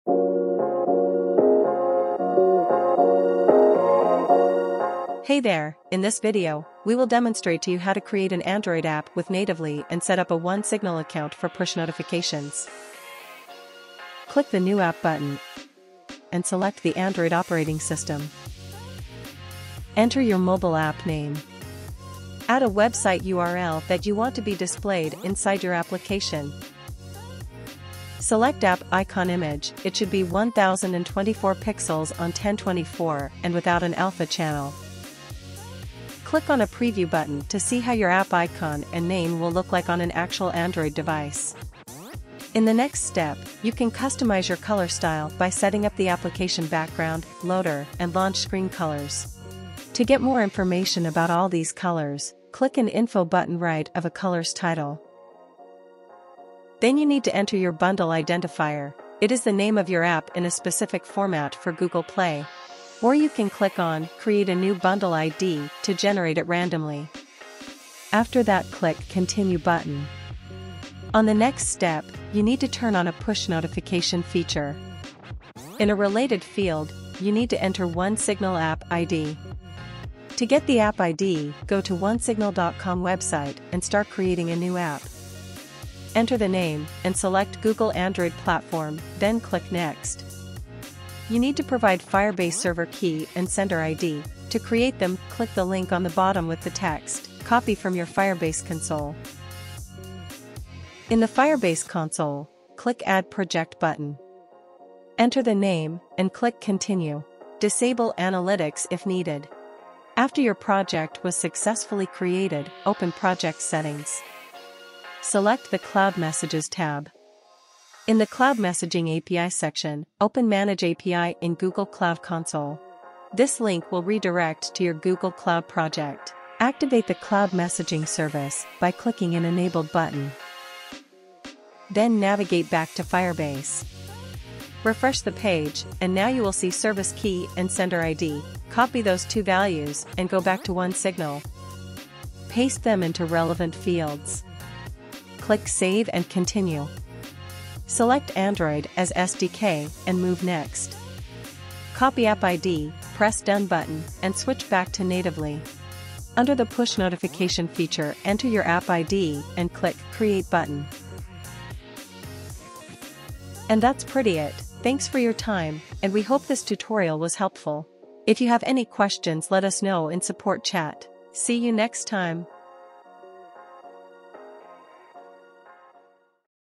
How to create (convert) an Android App with Natively and set up OneSignal Push Notifications
Natively is a no-code development tool that helps you create mobile applications in a few minutes.

Natively wraps(converts) your website/web app to the native mobile app by adding it to the WebView (an embeddable browser that a native application can use to display web content while a web app provides additional functionality and interactivity).

You can easily access mobile native features (such as push notifications, QR/bar code scanner, toast, banners, geolocation, camera, etc.) with our SDK & Plugins and continue updating your website without re-submitting it to AppStore/GooglePlay.

This video shows how to convert a website to an Android App and set up One Signal notifications using Natively.

with ❤️ from 🇺🇦